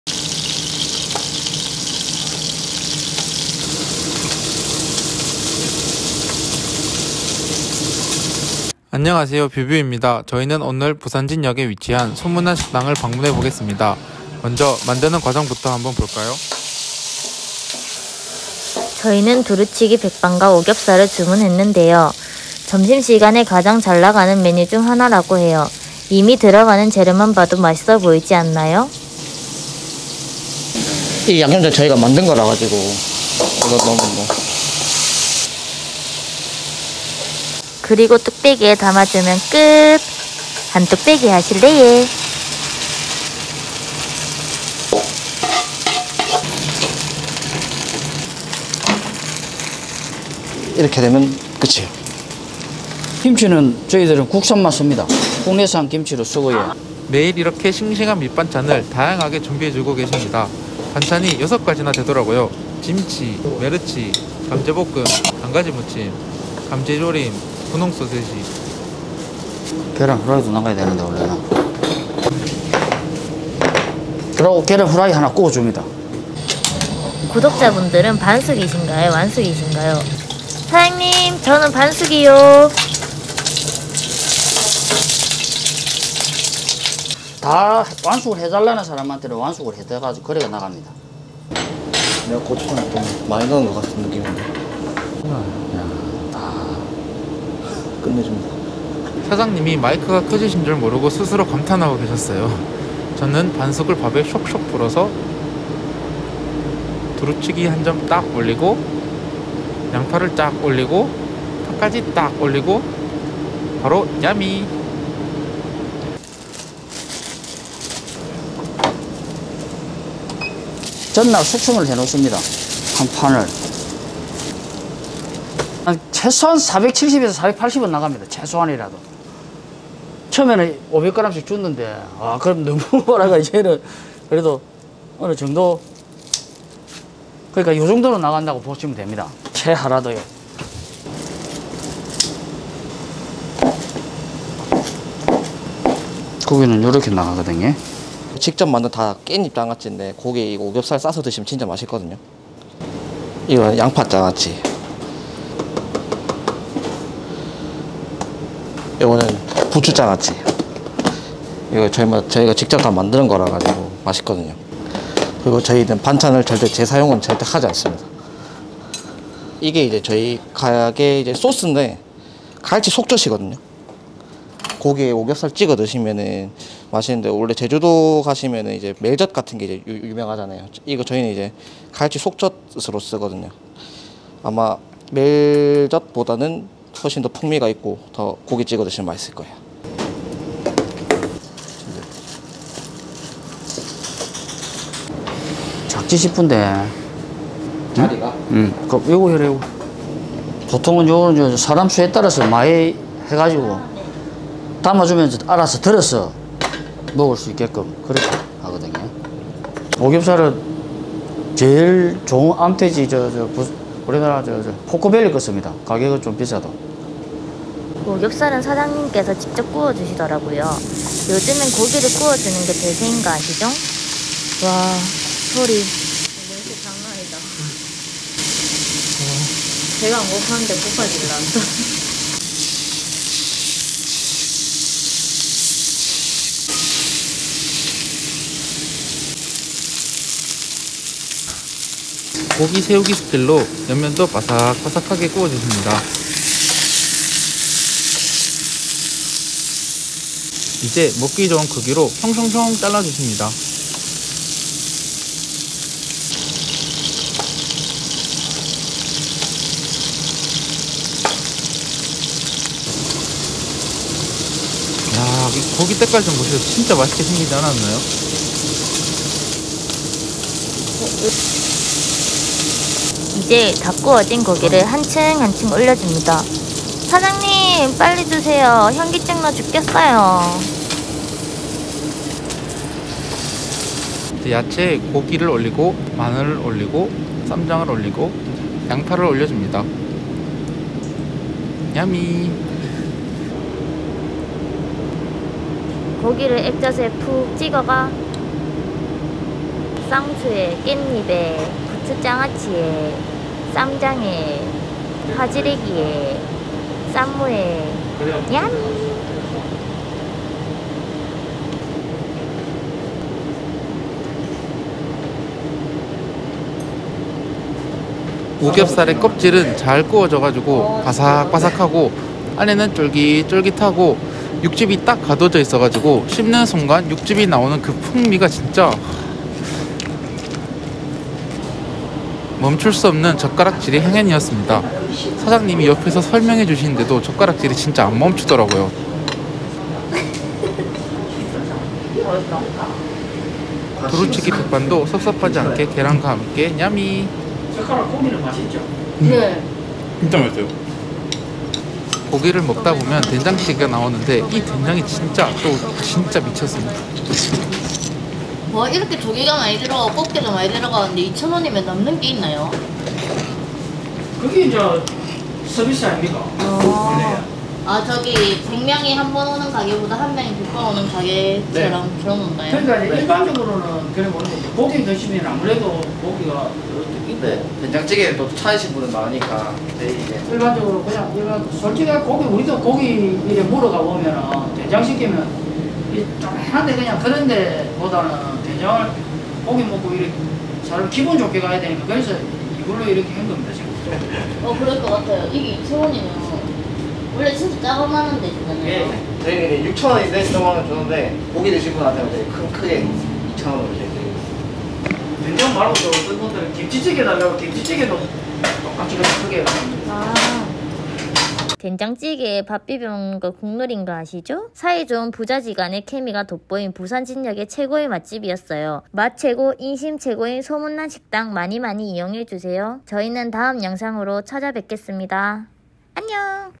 부산,수정동 근본 진짜 오겹살 맛집! 혼자 알기 아까워 공유합니다!/best pork belly restaurant! I don't want to share it with you.
부산진역에 위치한 오겹살 근본 맛집
더해서 남자들의 소울푸드 두루치기까지?!
다양한 메뉴와 혜자스러운 구성으로
혼자 알기 아까워서 공유합니다
'소문난 식당'

제발 가보세요!!

 🍴 메뉴

- 오겹살 1인 세트 : 13,900원

- 된장찌개 : 6,000원

- 김치찌개 : 6,000원

- 두루치기백반 : 7,000원

- 수제돈까스 : 6,500원

- 갈비탕 : 7,500원

- 육개장 : 7,000원

- 낙지덮밥 : 7,000원

- 오므라이스 : 6,000원

🔎 위치 & MAP - 부산 동구 수정로 22 1층 소문난 식당